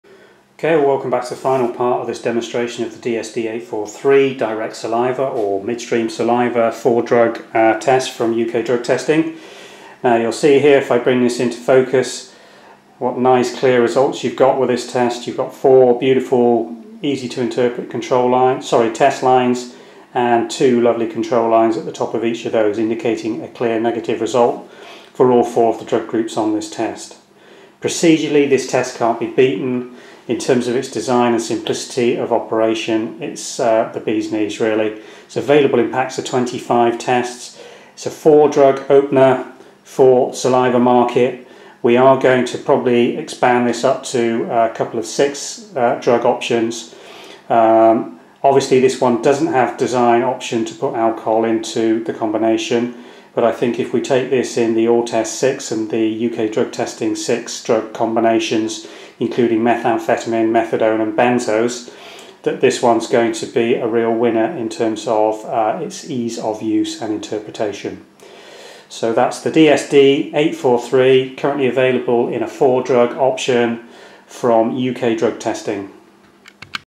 DSD-843 saliva drug test results part 3
Reading the results of an oral fluid saliva drug test kit. This video demo shows the results and how to read them. Information on drug testing issues, oral fluid drug testing kits and instructions provided